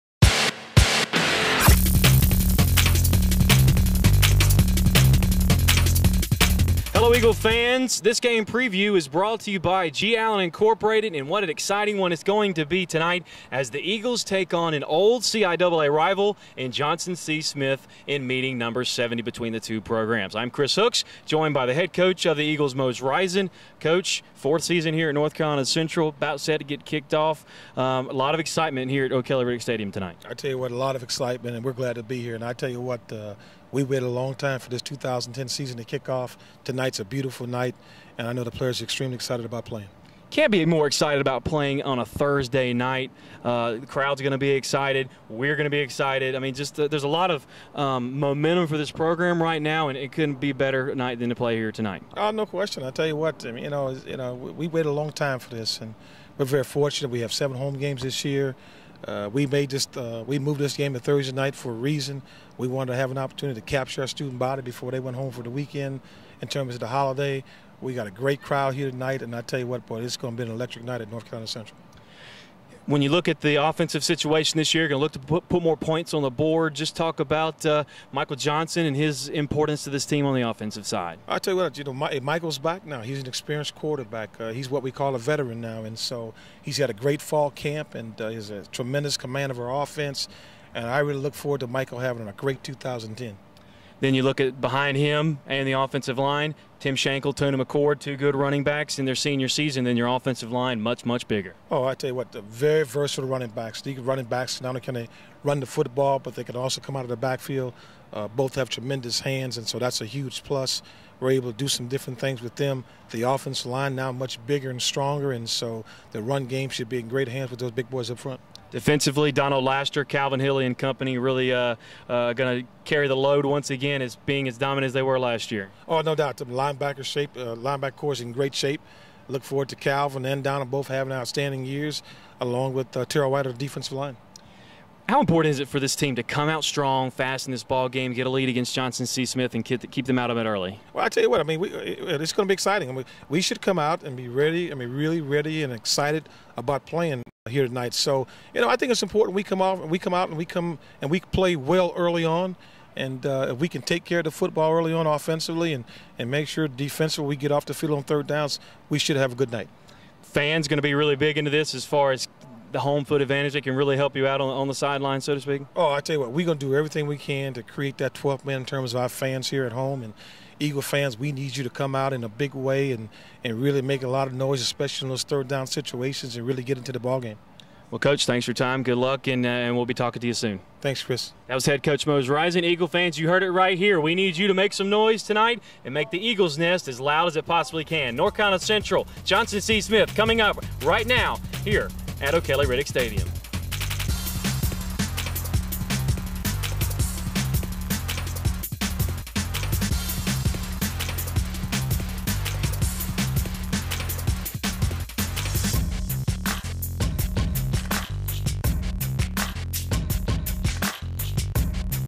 G. ALAN INC. PREVIEW JOHNSON C. SMITH
G. Alan Incorporated give you an inside look to the 70th gridiron matchup between North Carolina Central and Johnson C. Smith.  Chris Hooks of the NCCU Sports Network talks to head coach Mose Rison to prepare Eagle fans for the 7:00 p.m. Thursday night kickoff to the 2010 Football Season at O'Kelly-Riddick Stadium.

G. Alan Incorporated, I'm with G.